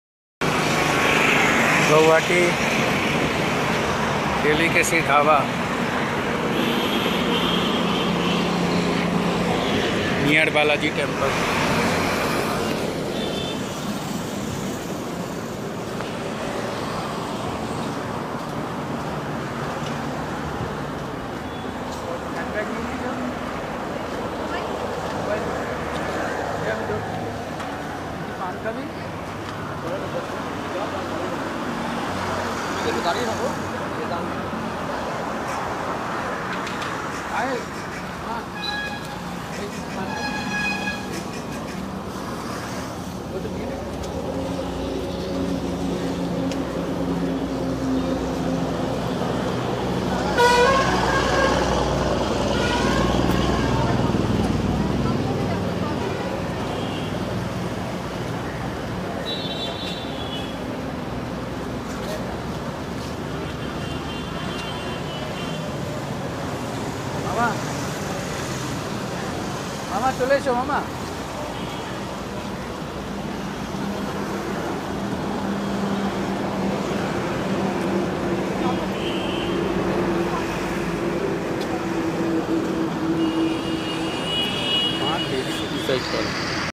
Delicacy Dhaba Guwahati near Balaji Temple on National Highway 37
Delicacy Dhaba Guwahati near Balaji Temple on National Highway 37 (NH-37) in Assam.

Delicacy Dhaba just in front of Hotel Royal Heritage opposite Balaji Temple on NH-37 (National Highway 37) in Guwahati.

Video showing the National Highway 37 (NH 37) road traffic condition, outside view of famous Delicacy Dhaba located close to Balaji Temple of Guwahati.

Delicacy Dhaba on NH 37 in Guwahati serves delicious Indian and Chineese dishes.